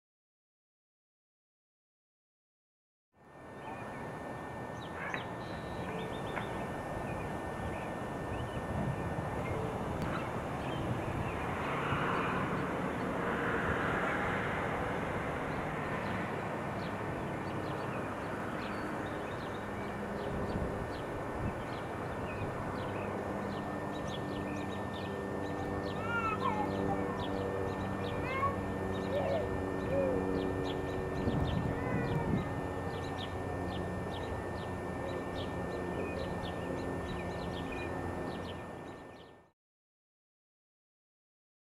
Neighborhood View
A slice of the neighborhood as seen and heard from my rooftop.  Timelapse video layered with audio clips from the original video.